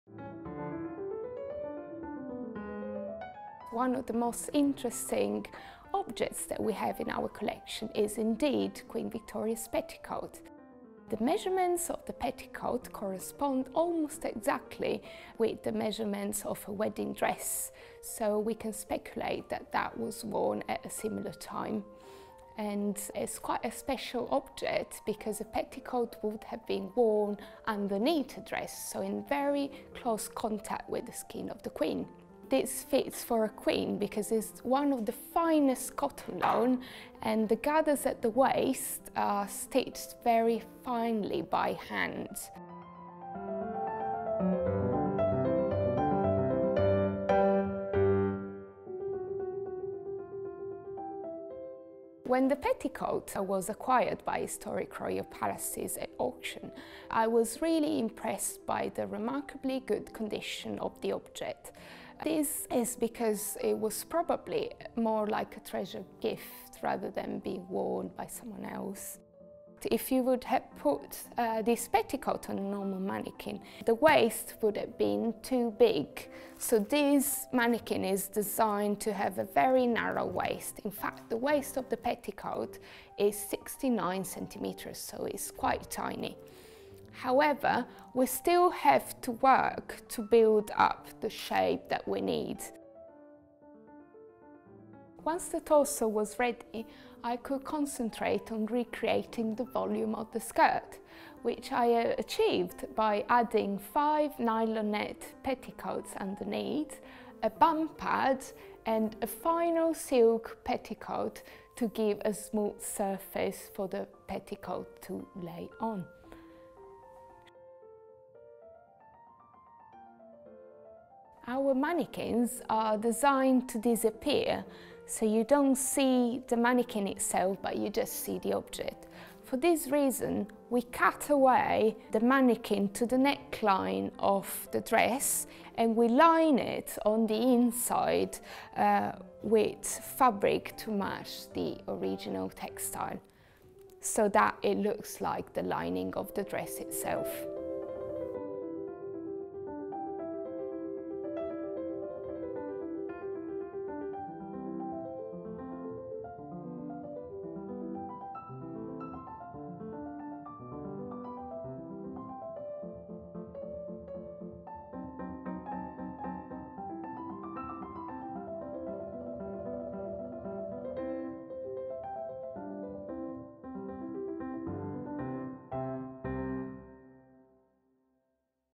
Queen Victoria's petticoat is installed at Kensington Palace
“The measurements of the petticoat correspond almost exactly to the measurements of her wedding dress…” Conservator Viola Nicastro describes the process of getting Queen Victoria’s delicate petticoat ready for display for Victoria200 at Kensington Palace.